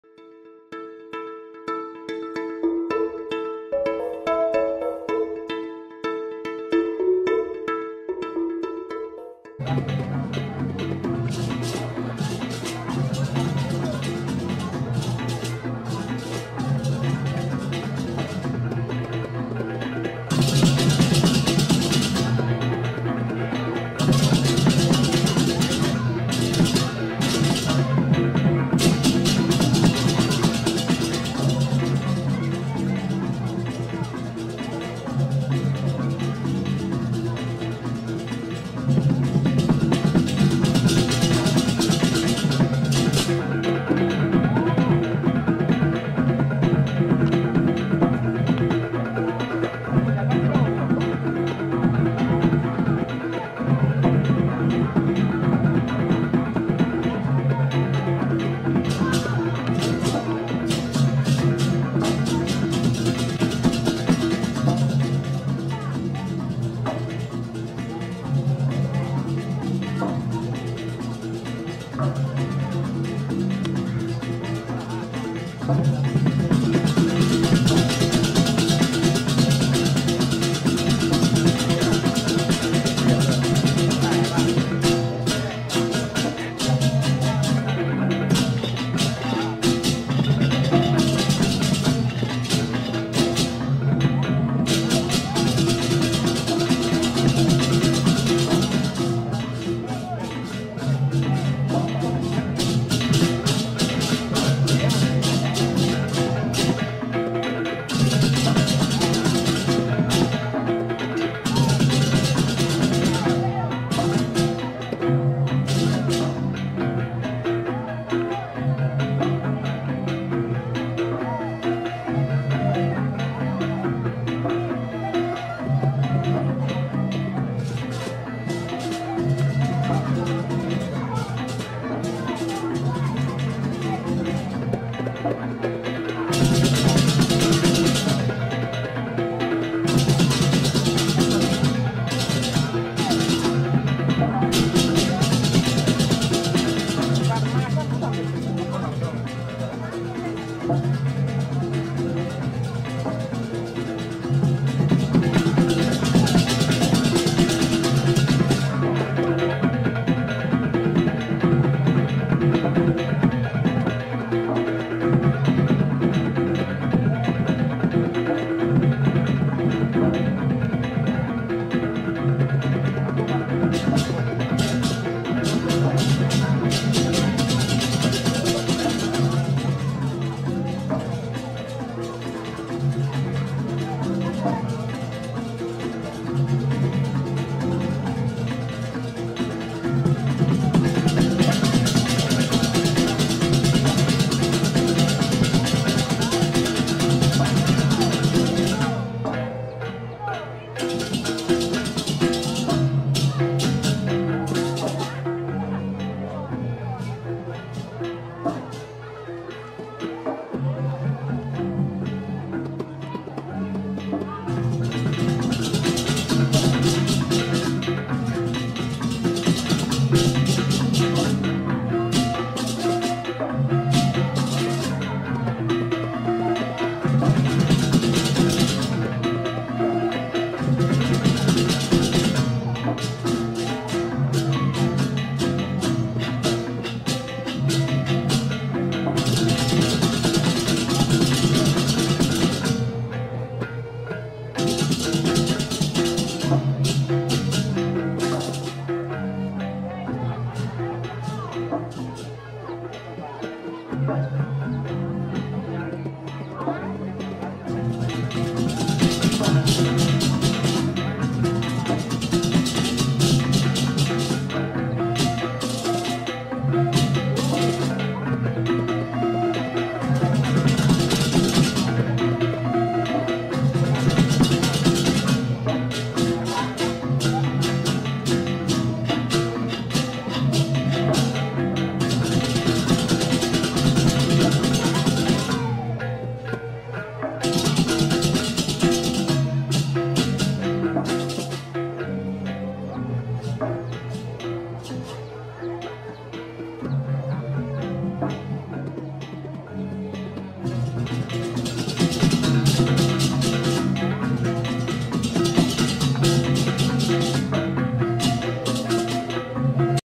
Proses Mendak Lembu Pelebon Griya Peling Padang Tegal
Proses mendak lembu dari Peliatam ke Griya Peling Padang Tegal Ubud